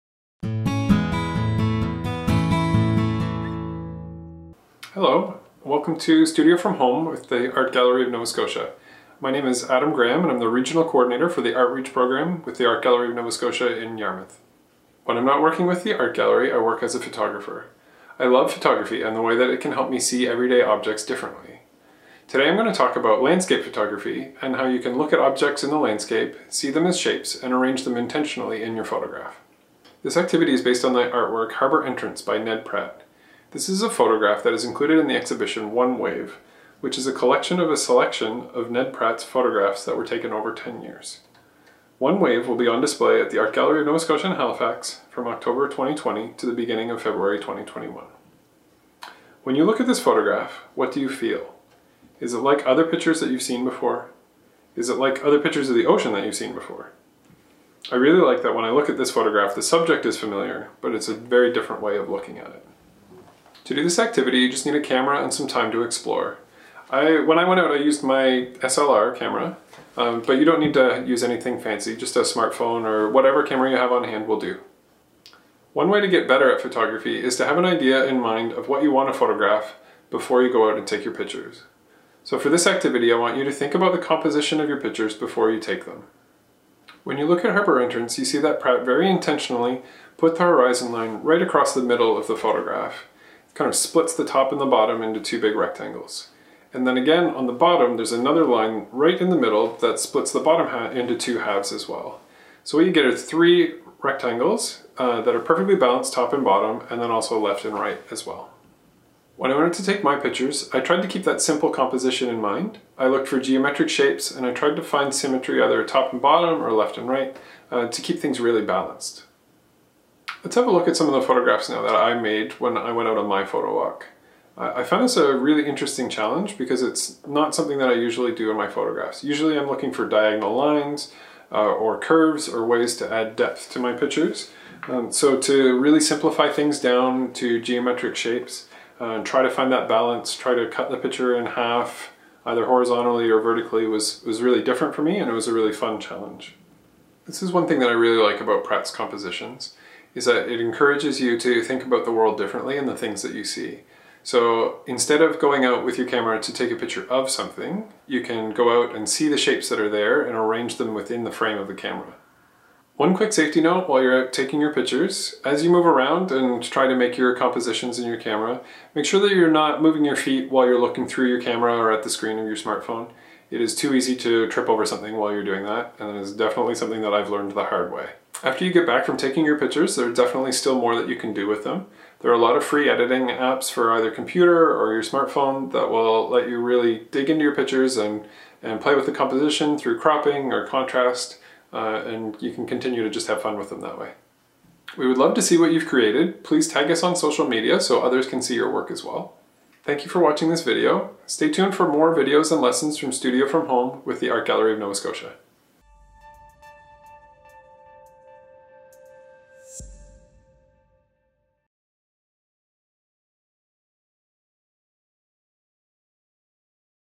Studio From Home: Compositions in Photography
This activity is based on the work Harbour Entrance by Ned Pratt which is included the exhibition Ned Pratt: One Wave, on display at the Art Gallery of Nova Scotia Halifax from October 2020 to February 2021. This piece is a part of a series of photographs selected from a decade of Pratt’s work. Harbour Entrance is a good example of Pratt’s compositional style, creating balance and symmetry through the isolation of geometric shapes.